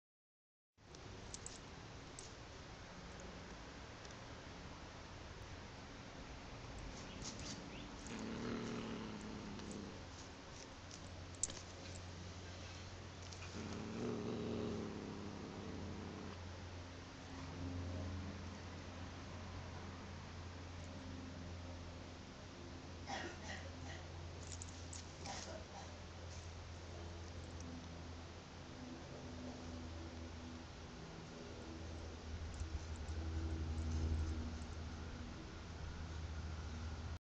Oska with a mouse
Oska and her first kill! Or maybe it was one of the other kitties, but it's hers now!